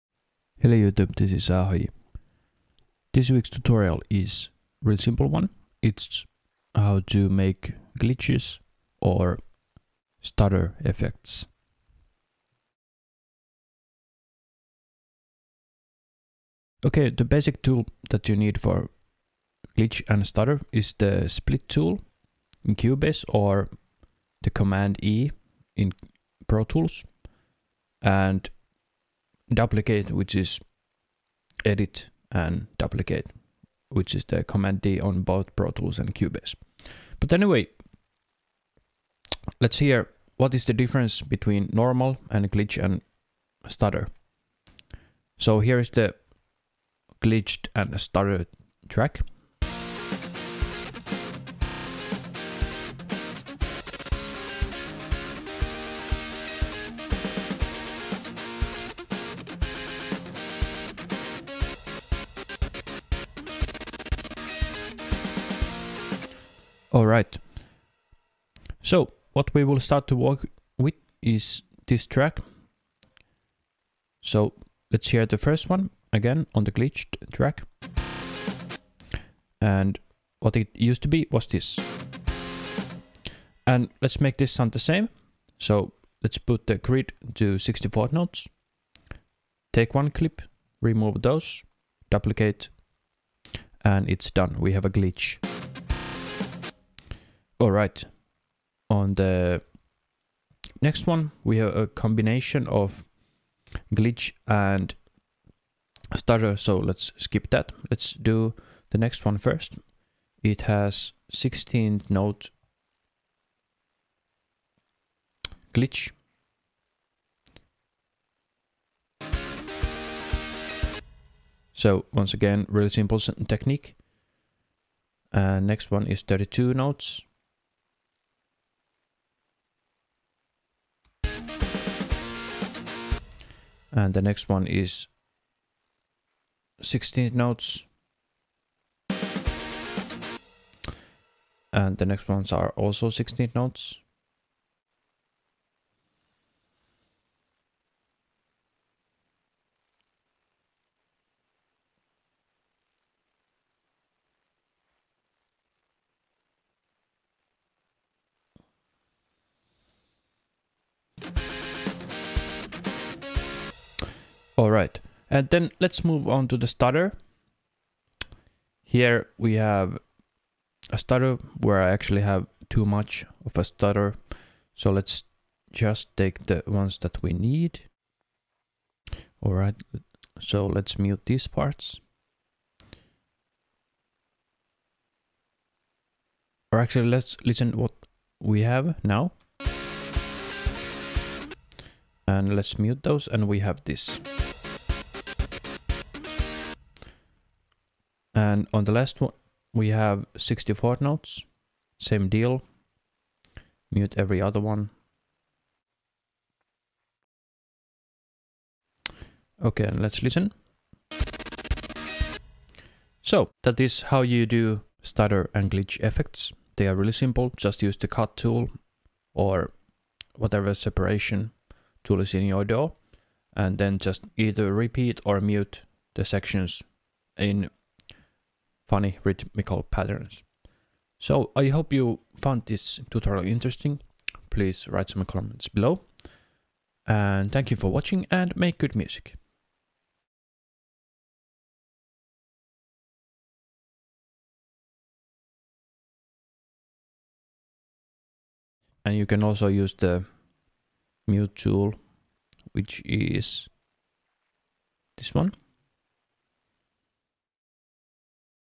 Editing 201 - Glitch and Stutter effect tutorial
Quick tutorial on the overused effects, but it's still good to know how to do them. Glitches are more effective on a bit more shorter and percussive stuff and stutters are more effective on long notes.